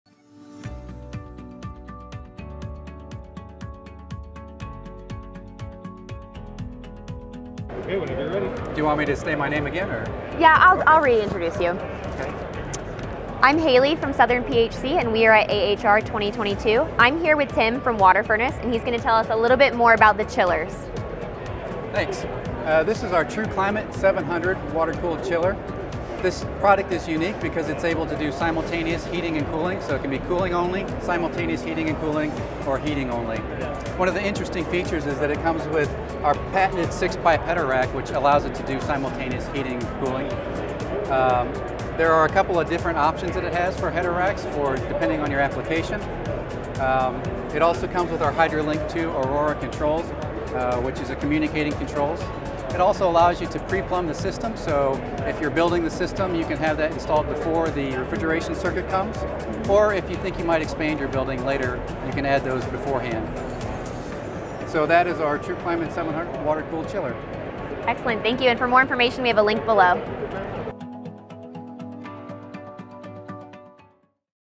WaterFurnace TruClimate 700 Water-Cooled Chiller at AHR Expo 2022
Tim from WaterFurnace introduces their TruClimate 700 Water-Cooled Chiller. This product is able to do simultaneous heating and cooling–so it can be cooling only, simultaneous heating and cooling, or heating only. It comes with our patented 6-pipe header rack, which allows it to do simultaneous heating/cooling. It also comes with their HydroLink2 Aurora Controls, which is a communicating control, and it allows you to pre-plumb the system.